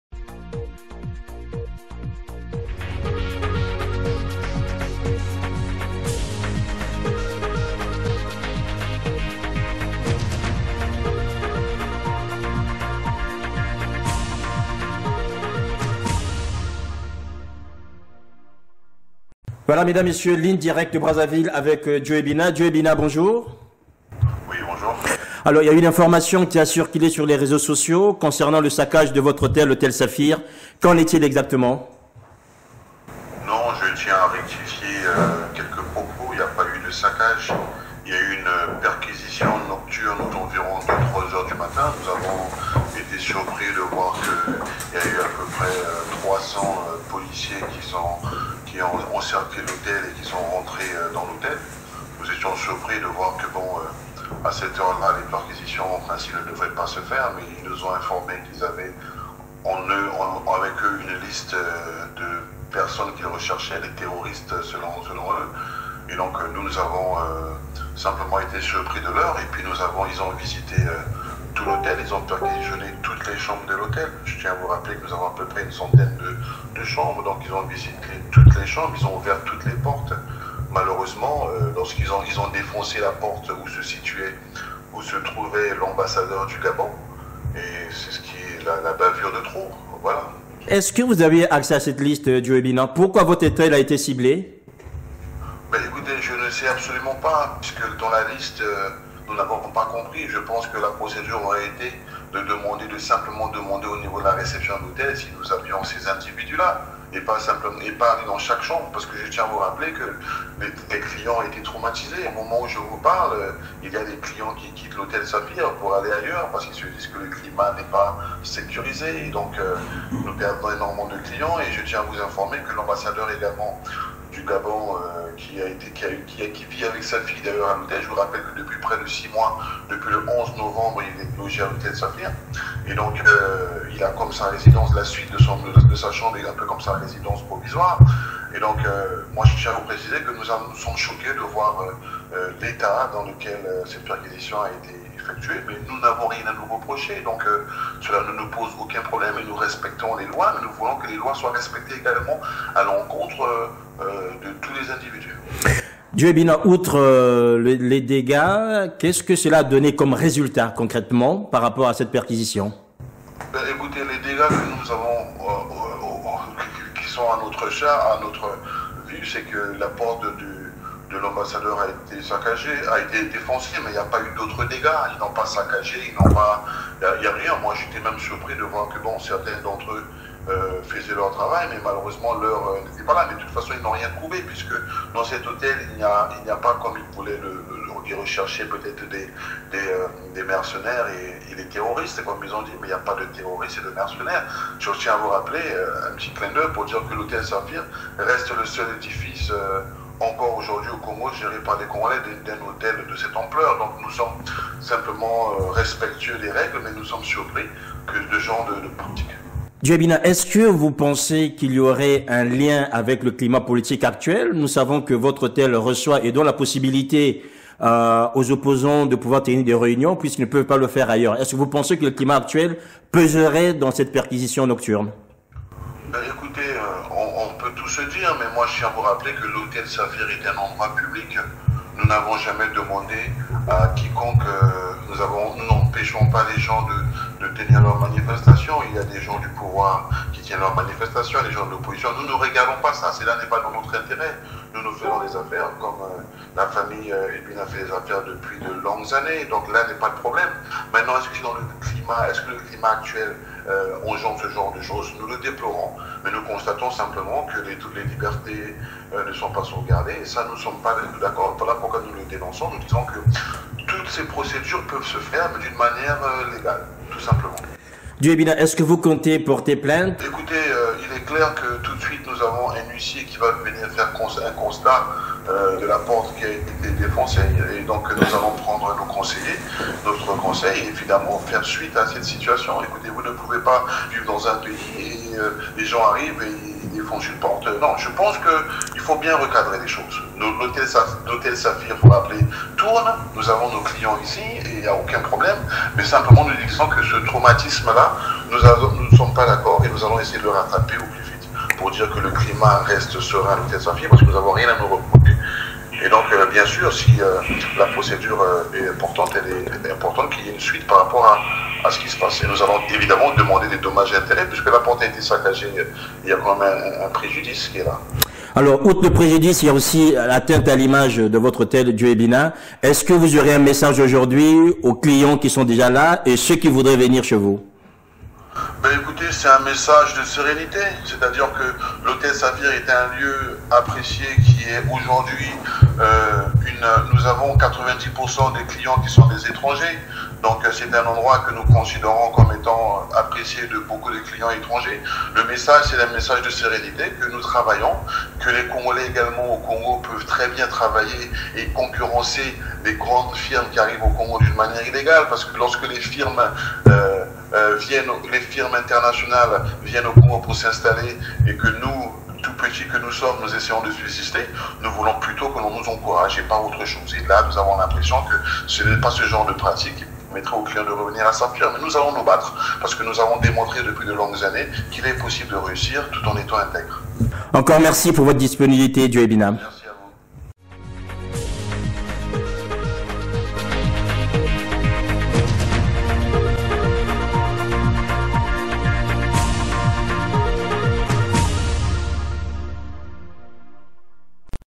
EXCLUSIF. Explications de Joe Ebina sur l'opération policière nocturne à l'Hôtel Saphir
Plus de 300 policiers armés ont perquisitionné l'Hôtel Saphir à 3h du matin dans la nuit du 1er au 2 avril 2016.